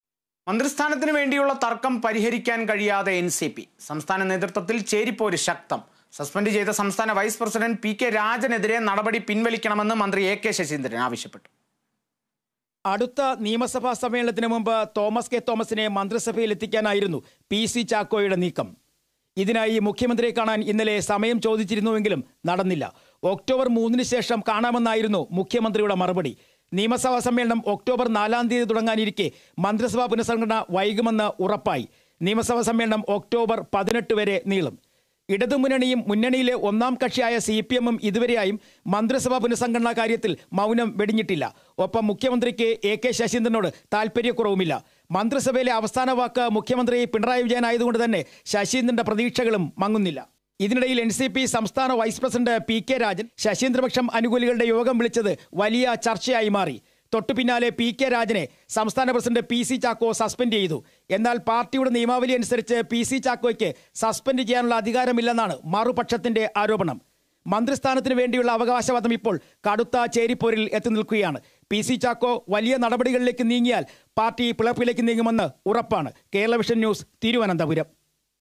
മന്ത്രിസ്ഥാനത്തിന് വേണ്ടിയുള്ള തര്‍ക്കം പരിഹരിക്കാന്‍ കഴിയാതെ NCP
മന്ത്രിസ്ഥാനത്തിന് വേണ്ടിയുള്ള തര്‍ക്കം പരിഹരിക്കാന്‍ കഴിയാതെ എന്‍സിപി. സംസ്ഥാന നേതൃത്വത്തില്‍ ചേരിപ്പോര് ശക്തം. സസ്‌പെന്‍ഡ് ചെയ്ത സംസ്ഥാന വൈസ് പ്രസിഡന്റ് പി കെ രാജനെതിരെയുള്ള നടപടി പിന്‍വലിക്കണമെന്ന് മന്ത്രി എകെ ശശീന്ദ്രന്‍ ആവശ്യപ്പെട്ടു.

About COA